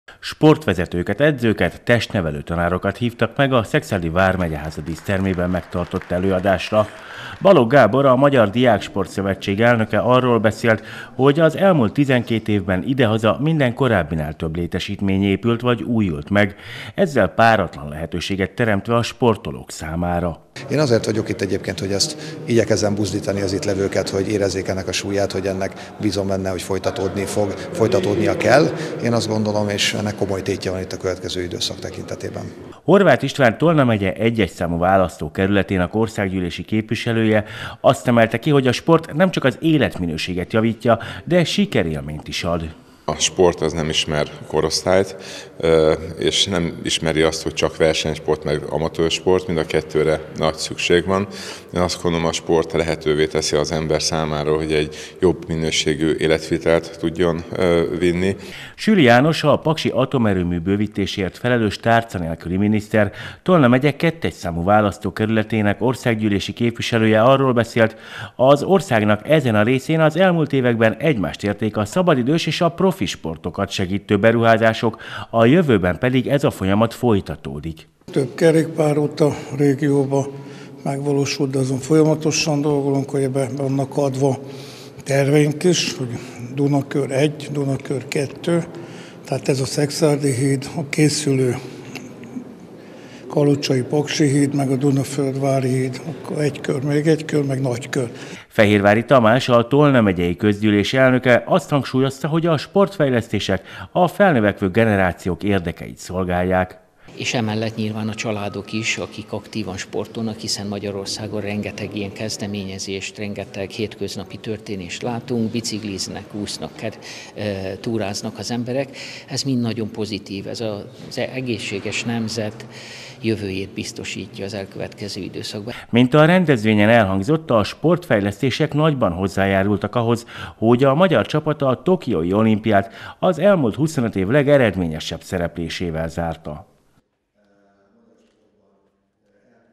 Sportkonferenciát tartottak Szekszárdon - bemutatták a fejlesztéseket
Az elmúlt évek legfontosabb hazai sportinfrastruktúra fejlesztéseit bemutató tájékoztató tartottak Szekszárdon. A rendezvényen Horváth István, Tolna megye egyes számú választókerületének országgyűlési képviselője arról beszélt, hogy ezek a beruházások nagyban hozzájárultak a lakosság egészségének megőrzéséhez.